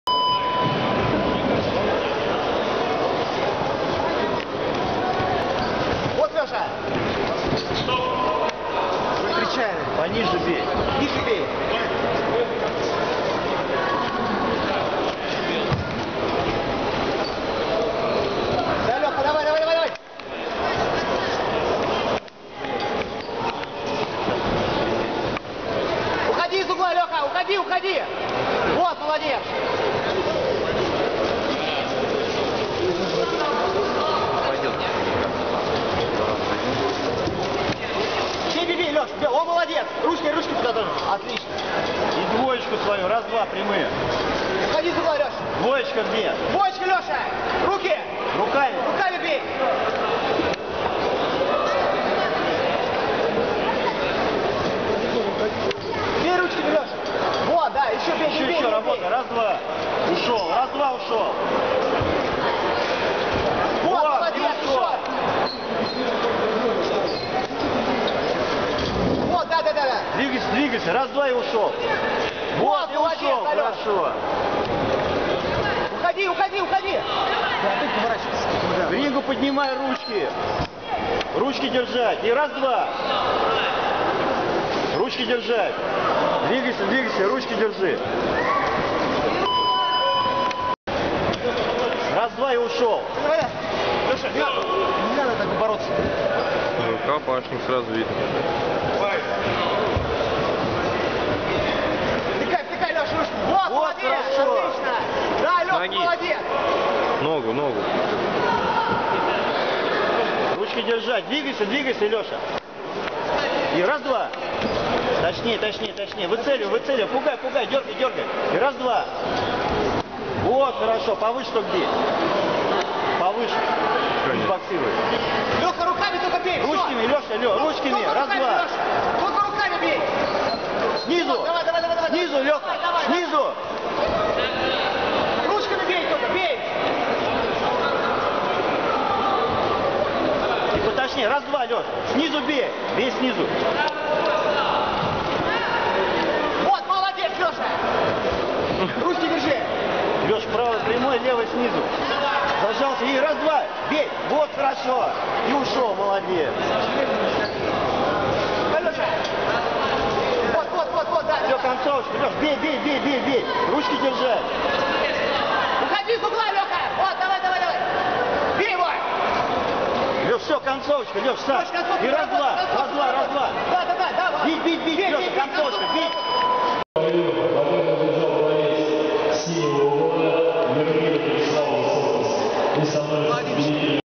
Спиридонов Алексей (красный угол).СПК "ЯРОПОЛК". Первенство ЦФО по кикбоксингу, апрель 2011 г.
В апреле 2011 года в г. Зеленоград прошло Первенство ЦФО по кикбоксингу среди юношей от 12 до 17 лет.
В соревнованиях приняло участие более 150 спортсменов из клубов Центрального федерального округа.
Спортсмены СПК "ЯРОПОЛК" заняли следующие места:
- Спиридонов Алексей 2 место
- Нечипоренко Дмитрий 3 место
Поздравляем наших ребят!